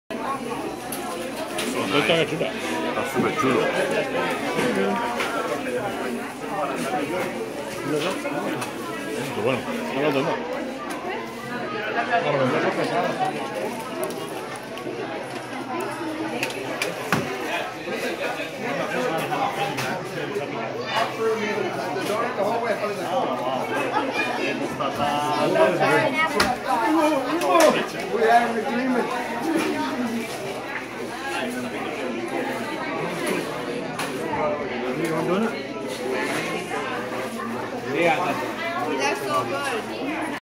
Post wedding 10/12/14
Tasting all the delicious fall treats